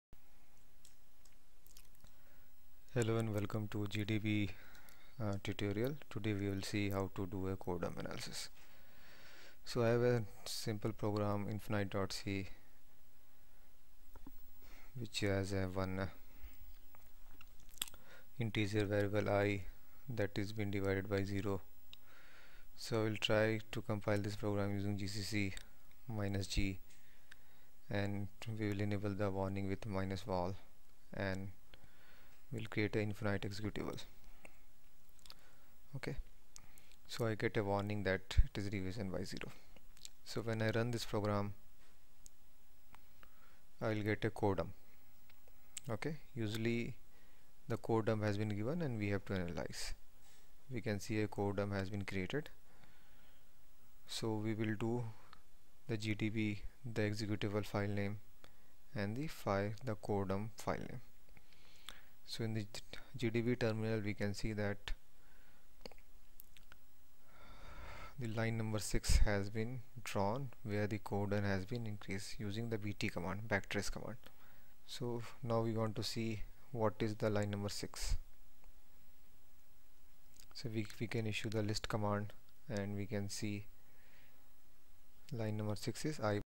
gdb core dump analysis for beginners
Debug the Core file using gdb, The GDB Core dump analysis
If a core file is created during a run of your program, you can open your debugger, say GDB, on that file and then proceed with your usual GDB operations
What is Core file
A core file contains a detailed description of the program's state when it died: the contents of the stack (or, if the program is threaded, the stacks for each thread), the contents of the CPU's registers (again with one set of register values per thread if the program is multithreaded), the values of the program's statically-allocated variables (global and static variables), and so on
gdb Core Dump Analyse für Anfänger
对于初学者gdb的核心转储分析
gdb analyse core dump pour les débutants